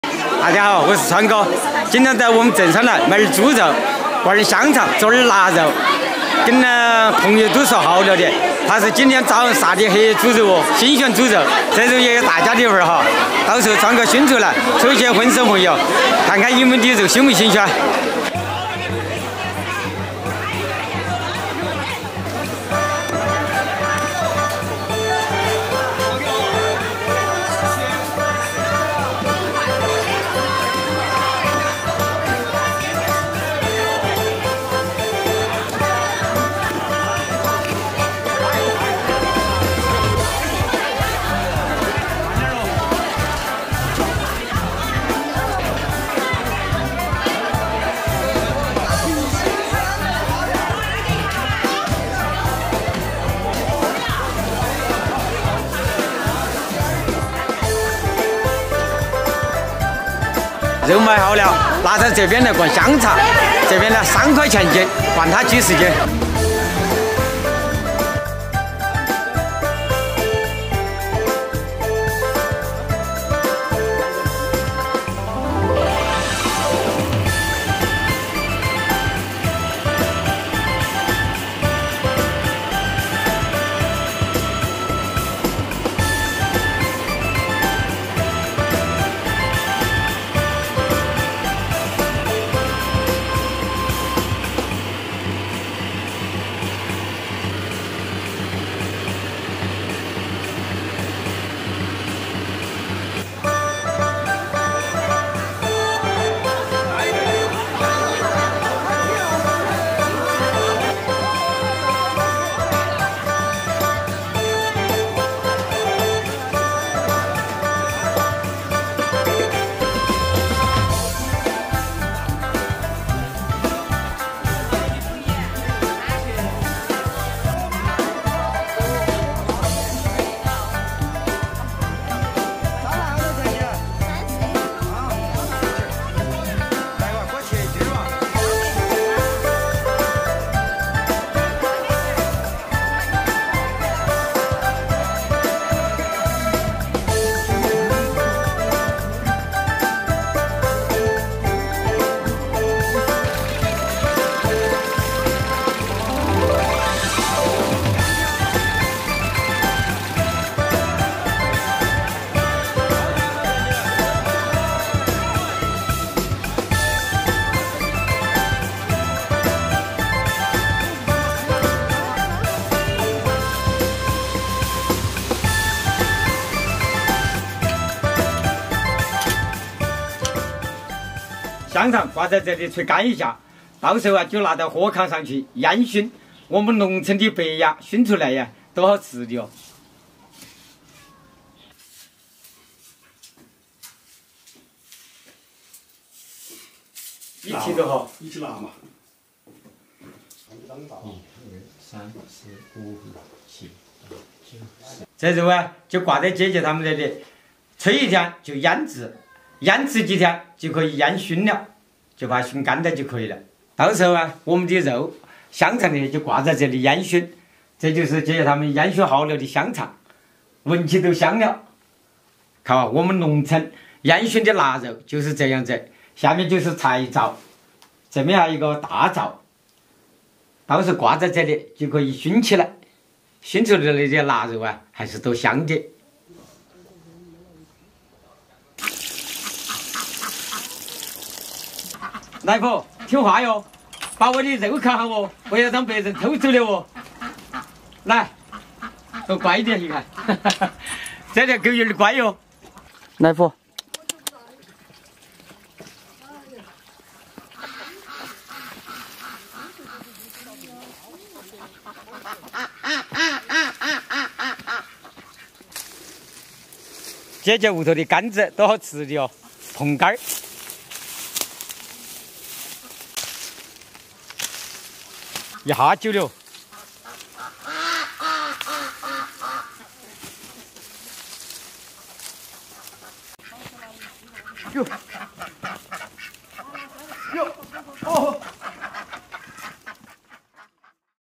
川哥买了2000元的猪肉，准备熏香肠腊肉，到时候抽一些粉丝朋友送【农民工川哥】
川哥 猪肉 香肠腊肉 粉丝朋友 请注意，本视频完全符合YouTube社区准则，没有任何违规行为。